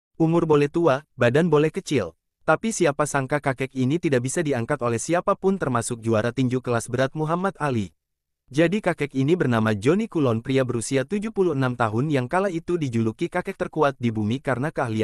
Johnny coulon kakek terkuat
Umur boleh tua,badan boleh kecil
tapi siapa sangka kakek ini tidak bisa diangkat oleh siapa pun termasuk juara tinju kelas berat Muhammad Ali
Jadi kakek ini bernama Johnny coulon pria berusia 76 tahun yang kala itu di juluki kakek terkuat di bumi karna keahliannya yang luar biasa
Hanya dengan menempelkan jarinya di leher pengangkatnya maka siapa pun gak bisa mengangkat kakek ini
Padahal kalian bisa lihat sendiri sebelumnya binaragawan ini dengan mudah mengangkat barbel Dengan sebelah tangan
Karna kemampuan nya tersebut banyak orang yang penasaran ingin mencoba mengangkat kakek ini mulai dari binaragawan kelas ringan sampai yang badan nya besar banget tetap gak bisa mengangkat kakek ini
Bahkan sekelas Muhammad Ali mencoba mengangkat kakek ini dan hasil nya tetap tidak bisa
Johnny coulon ni pada akhirnya meninggal dunia pada tahun 1973 yang usianya yang ke 84 tahun.